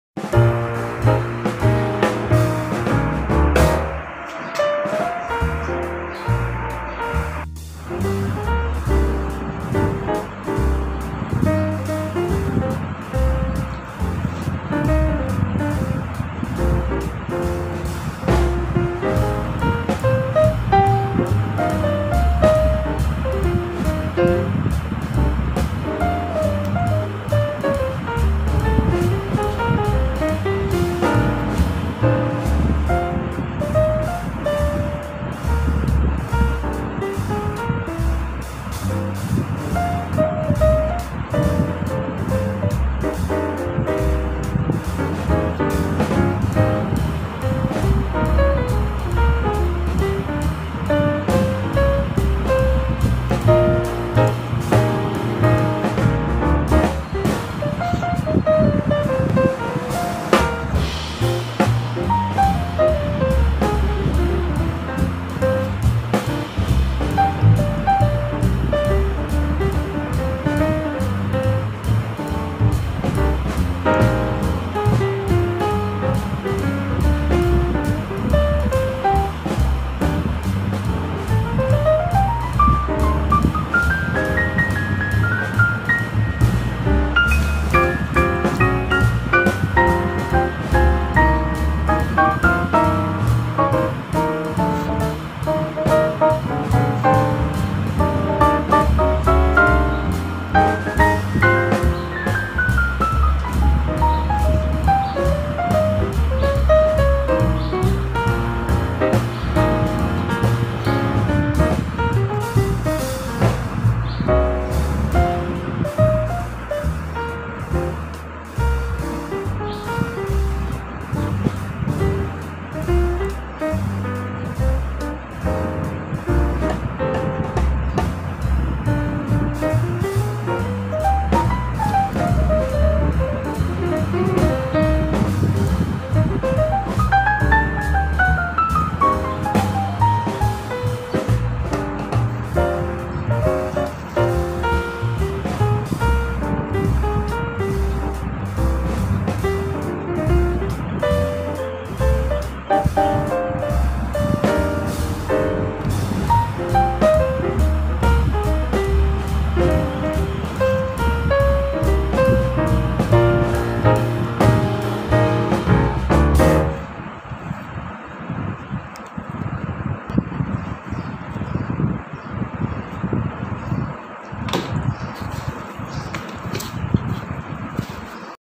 How to make dog with Cement // cement sculpture making step by step part 1
How to make dog with  Cement  // cement dog making step by step part 1